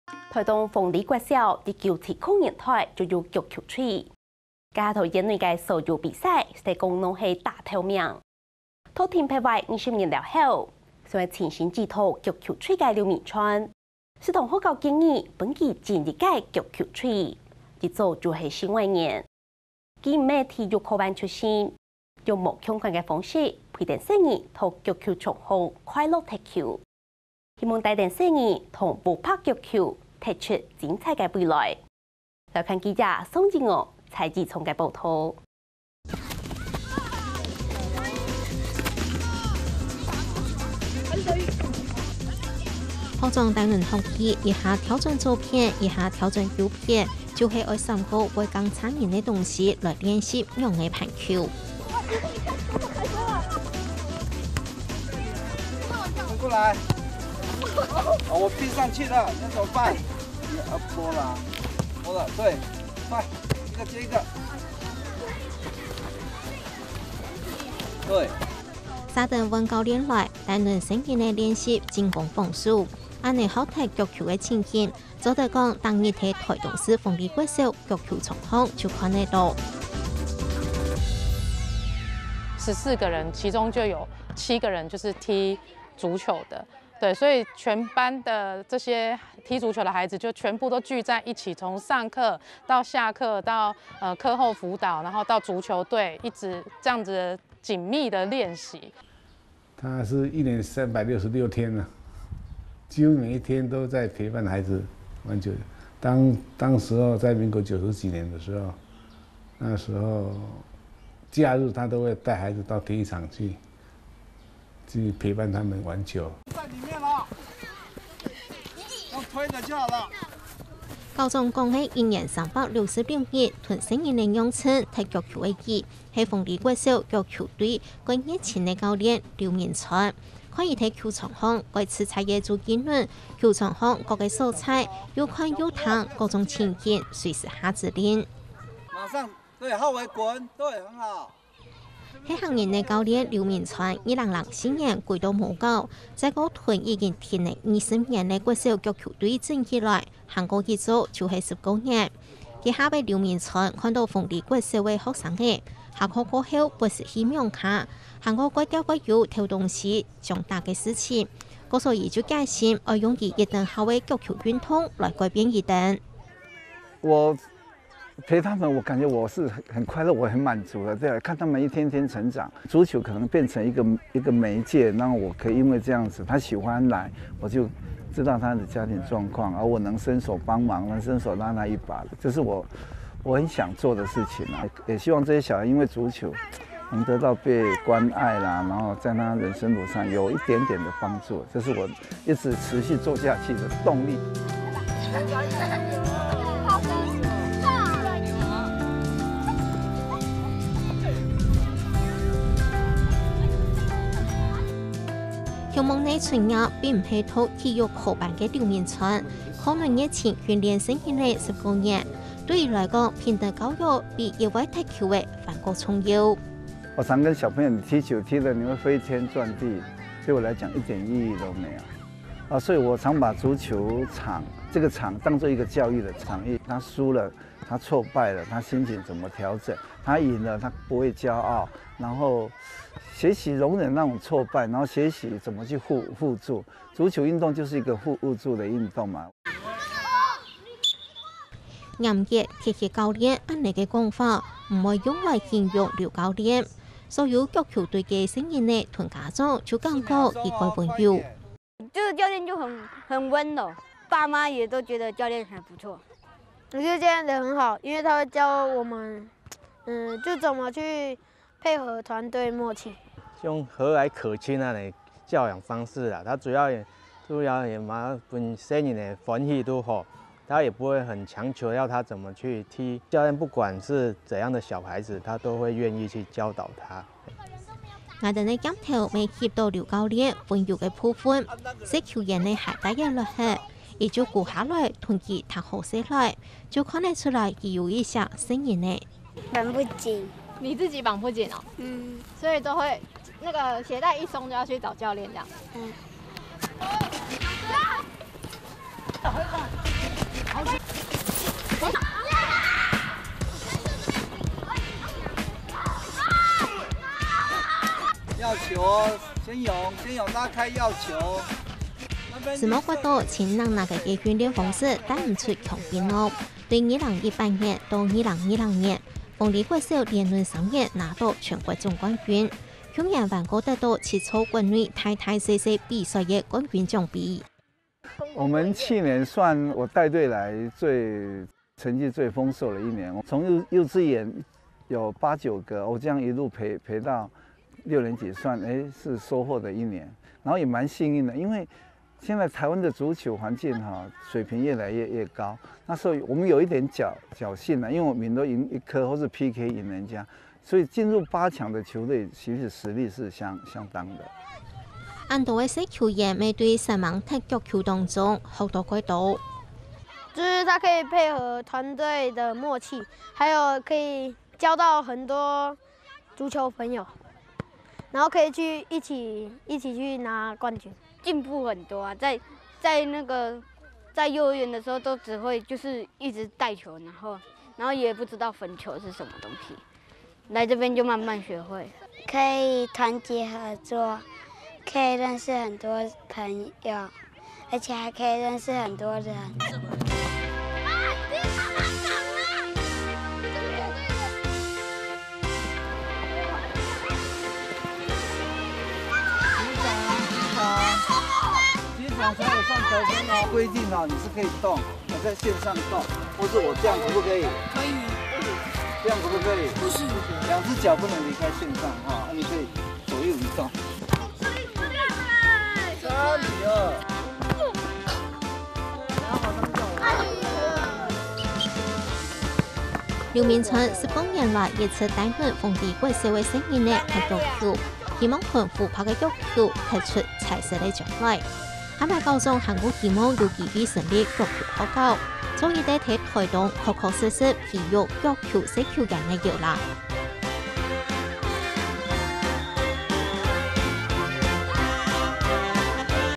《帶孩子踢出彩色未來》客家新聞雜誌第743集
台東豐里國小一九七○年代就設有足球隊，當時幾乎囊括所有縣內比賽冠軍，在中斷近20年後，懷抱親掌足球兵符夢想的劉明川，向母校毛遂自薦重組足球隊，一做就是十幾年。不是體育科班出身的他，跳脫制式訓練模式，陪著孩子在足球場上快樂奔馳，希望帶著孩子將黑白足球踢出彩色未來。記者宋宇娥跟蔡裕昌的報導。